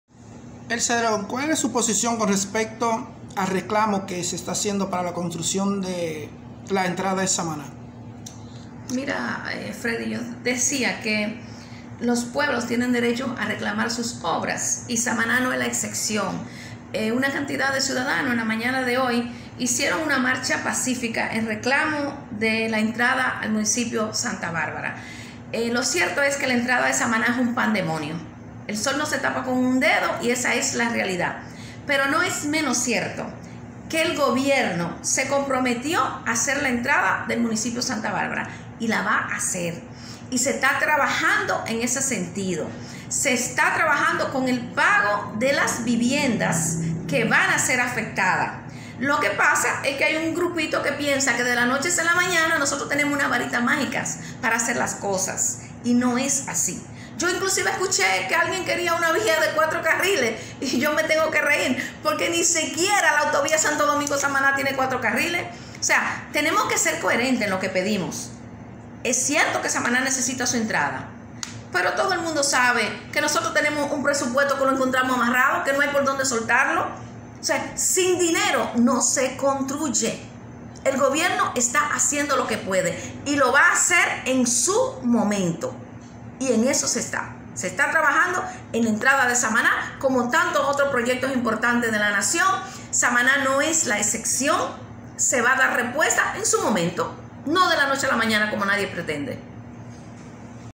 Gobernadora Elsa de León, "El gobierno se comprometió hacer la entrada de Samaná y la va hacer".
Hoy en Samaná se realizó una protesta  en reclamo por la contrucción de la entrada del municipio cabecera Santa Bárbara de Samaná.

Cierto de personas marcharon de manera pacífica por la calle principal, y en el frente de la gobernación, dónde fueron atendido por la gobernadora Elsa de León, la cuál afirmó, que el
 gobierno, se comprometió hacer la entrada de Samaná y la va hacer; dijo, "se está trabajando para realizar pago a los propietarios de las viviendas".

Los manifestantes reclaman la construcción de una carretera de 4 vía.

Tendremos más detalles hoy,  Por  DE INTERÉS TV, YouTube, siguenos.